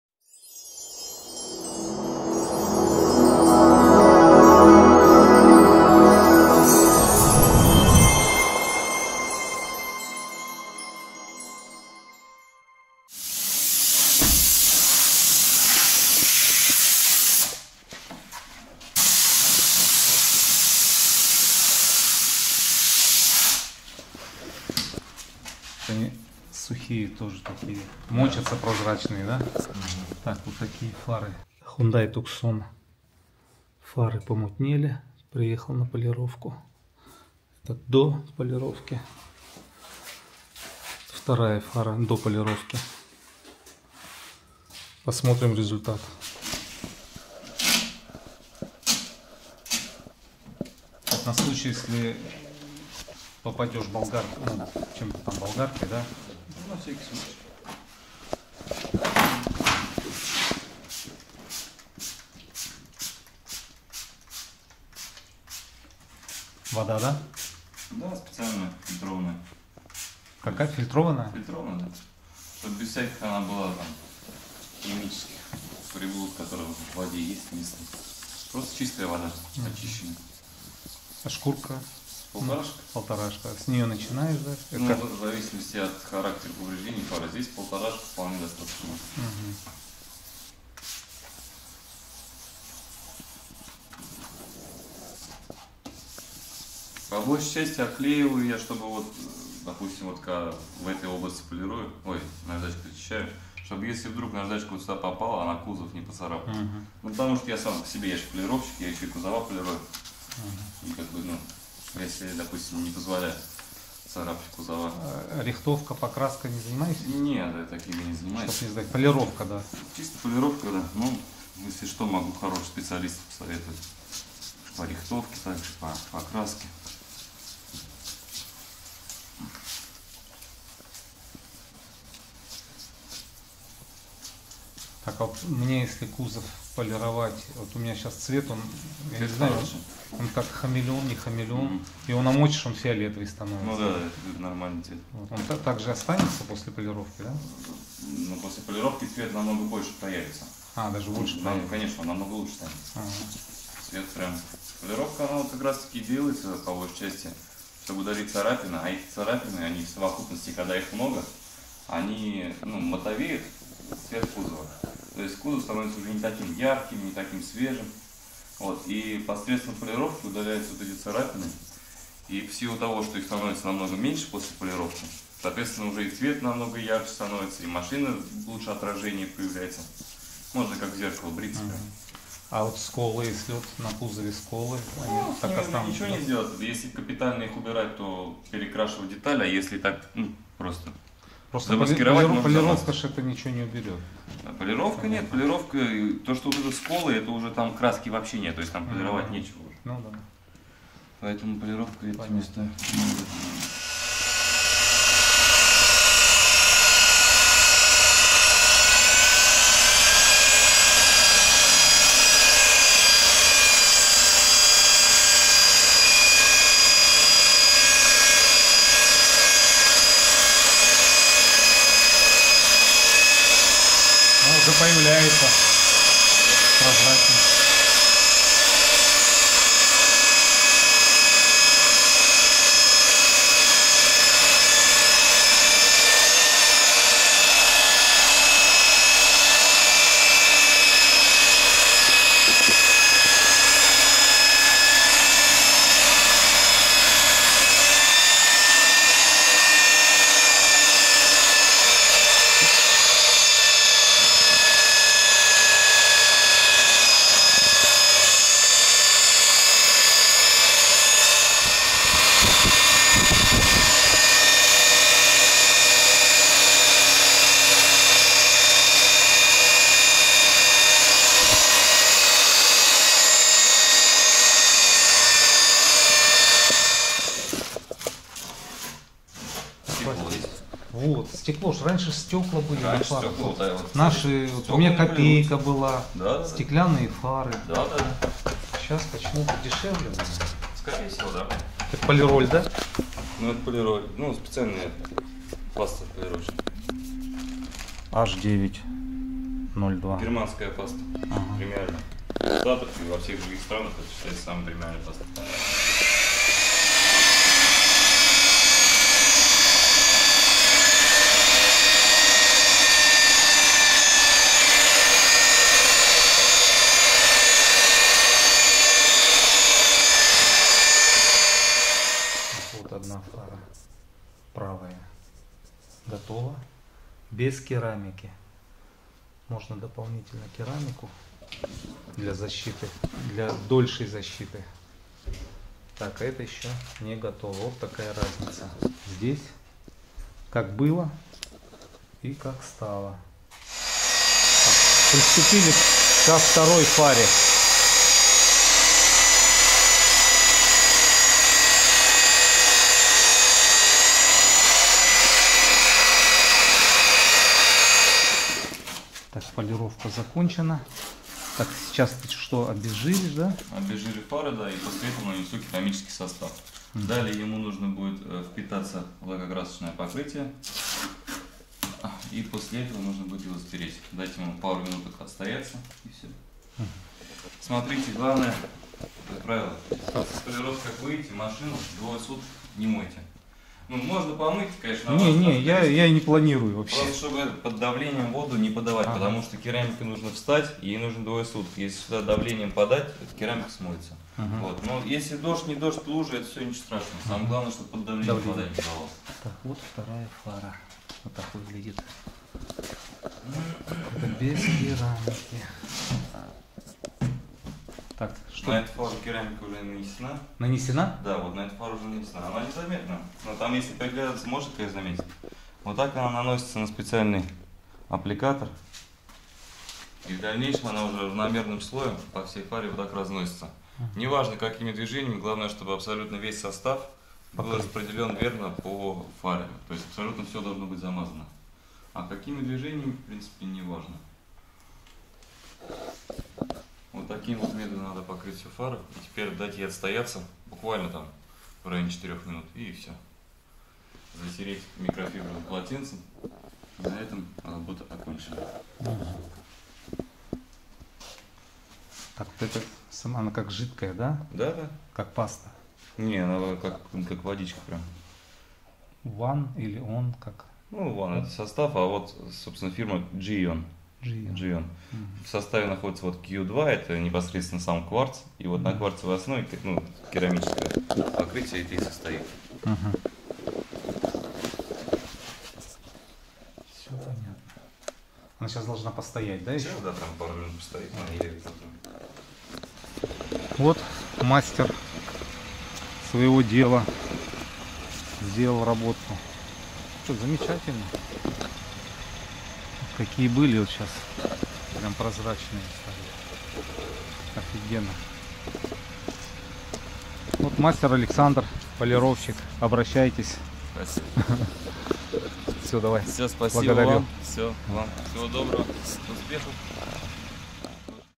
Полировка фар и кузовов в Астрахани. 28.11.2024.
Полировка фар и кузовов, покрытие защитным слоем керамики для продления срока службы. Мастер полировщик Александр, телефон(WhatsApp) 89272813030.